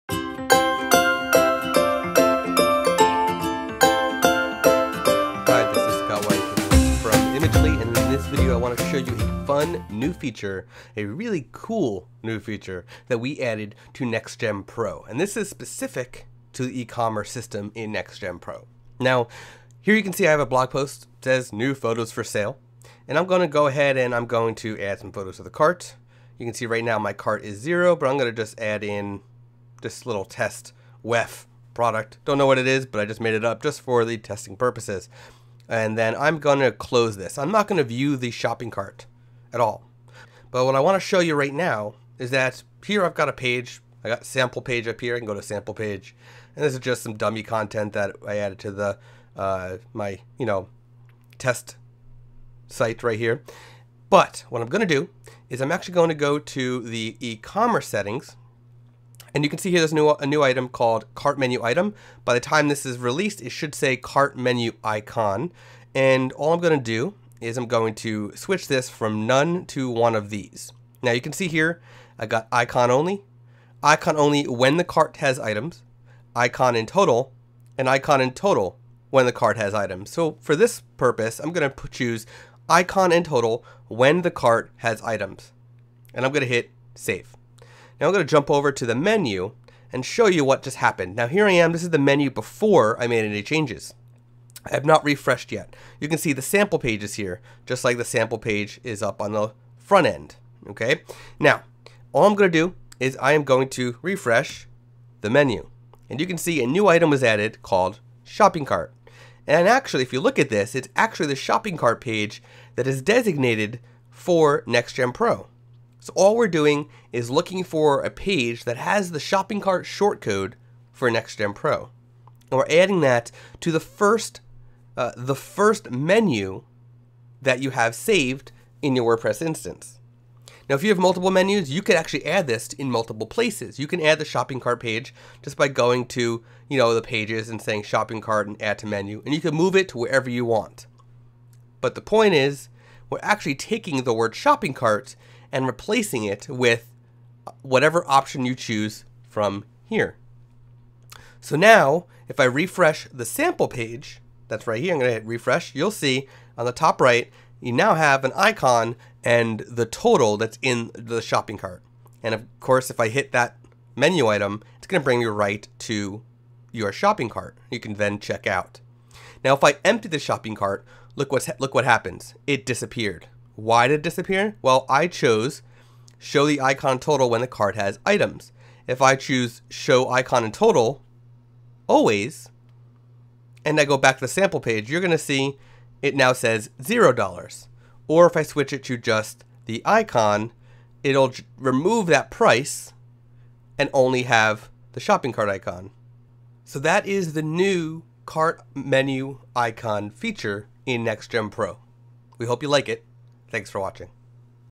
NextGEN Pro's Cart Menu Icon
The new Cart Menu Icon feature inside of NextGEN Pro.